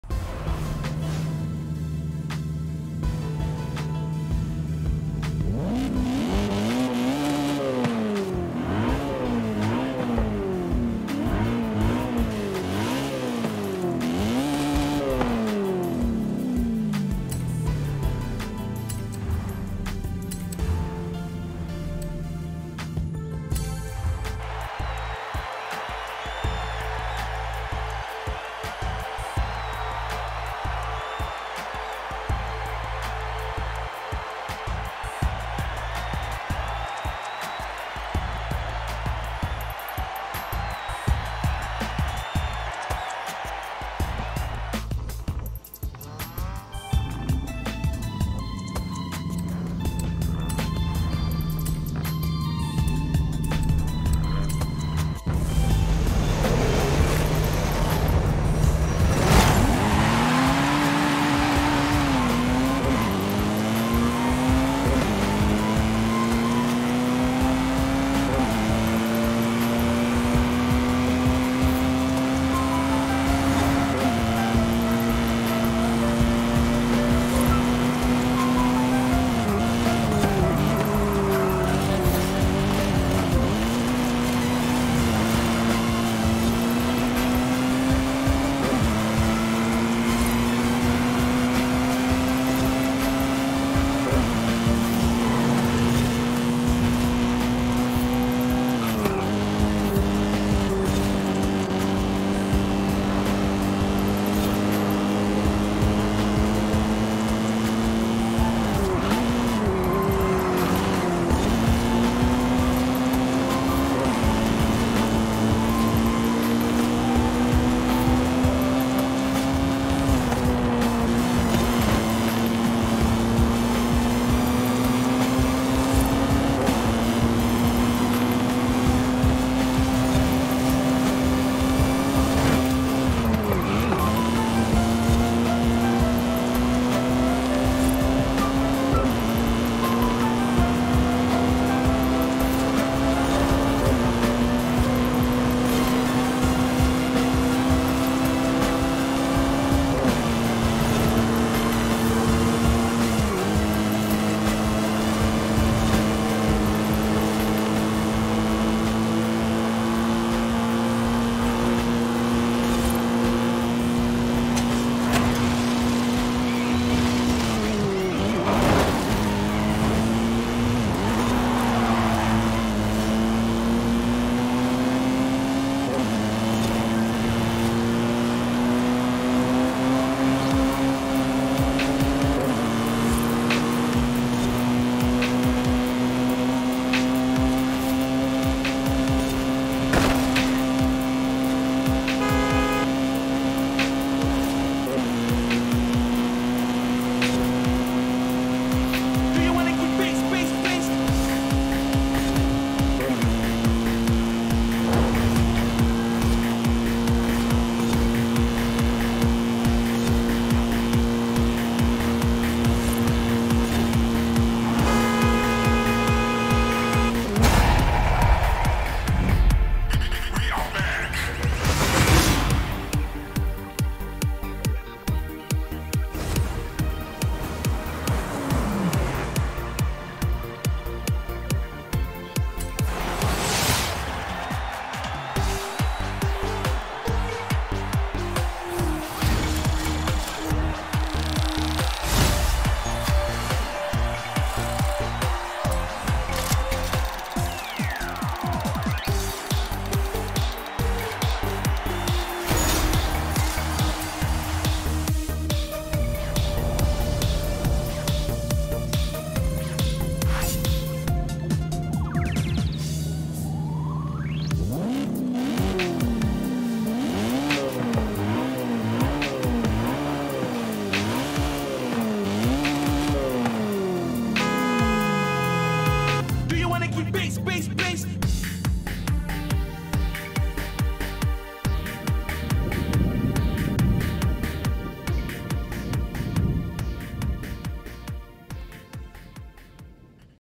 BMW M3 GTS
Beautiful BMW E92 M3 GTS from 2010. Car have 4.4L V8 engine producing 444 HP and 440 Nm of Torque with 7 speed transmission. Car is RWD with a weight of 1537 kg. I will Race on PRO difficulty on Ruta Norte course. Cheers.